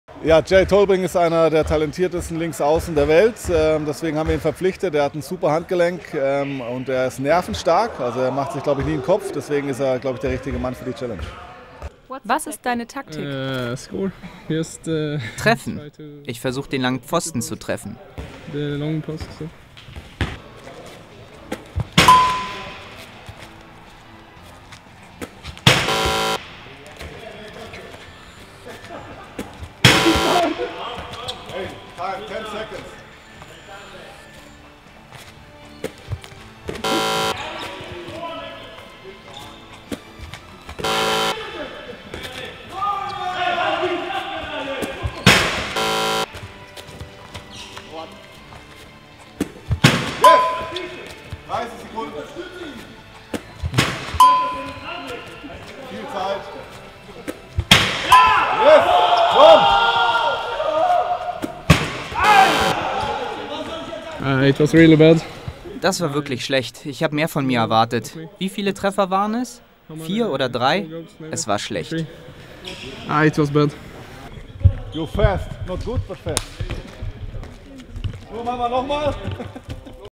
Clubchallenge 2017/18: Rhein-Neckar Löwen
Jerry Tollbring tritt für die Löwen zur Clubchallenge an.